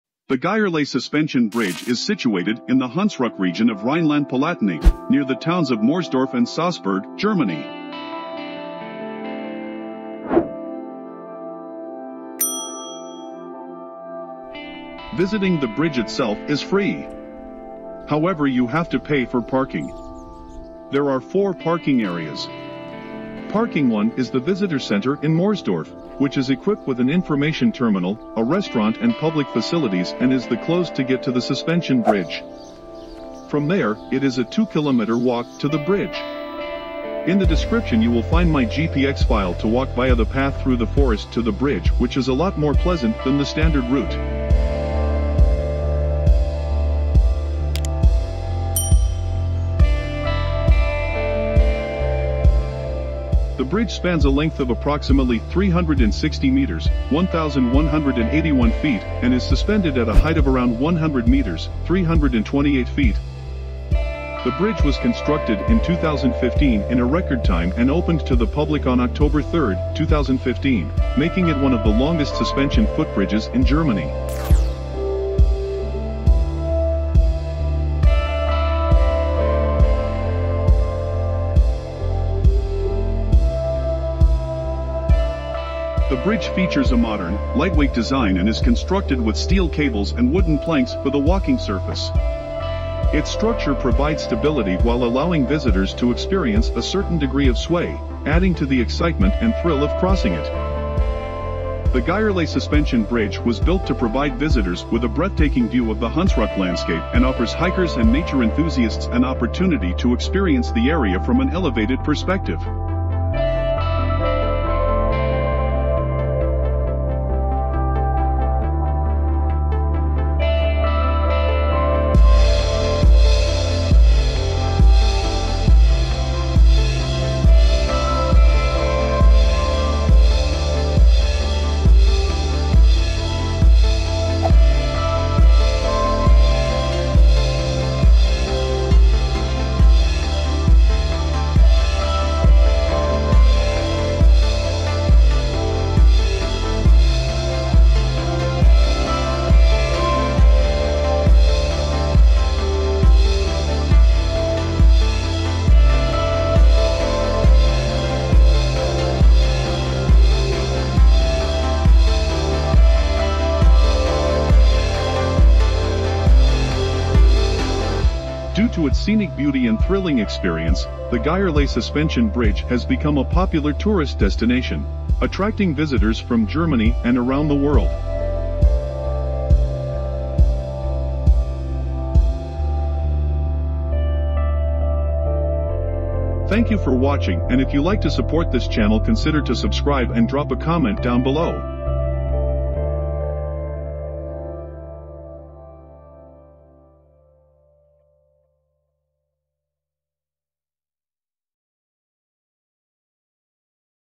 Geierlay Suspension Bridge | One of the LONGEST Suspension Bridges in Germany | Hängeseilbrücke
The Geierlay Suspension Bridge is a suspension bridge spanning the Moselle River valley in Germany.
It is located in the Hunsrück, a low mountain range that is part of the Rhineland plateau.
At 360 meters long, it is one of the longest suspension bridges in Europe.
The bridge was opened to pedestrians in 2015 and has since become a popular tourist destination.

Die Geierlay-Hängebrücke ist eine Hängebrücke, die das Moseltal in Deutschland überspannt.
Sie befindet sich im Hunsrück, einem Mittelgebirge, das Teil der Rheinischen Tiefebene ist.
Mit einer Länge von 360 Metern ist sie eine der längsten Hängebrücken in Europa.
Die Brücke wurde 2015 für Fußgänger geöffnet und hat sich seitdem zu einem beliebten Touristenziel entwickelt.

De hangbrug Geierlay is een hangbrug over de vallei van de rivier de Moezel in Duitsland.
Hij ligt in de Hunsrück, een middelgebergte dat deel uitmaakt van de hoogvlakte van Rijnland.
Met een lengte van 360 meter is het een van de langste hangbruggen in Europa.
De brug werd in 2015 opengesteld voor voetgangers en is sindsdien een populaire toeristische bestemming geworden.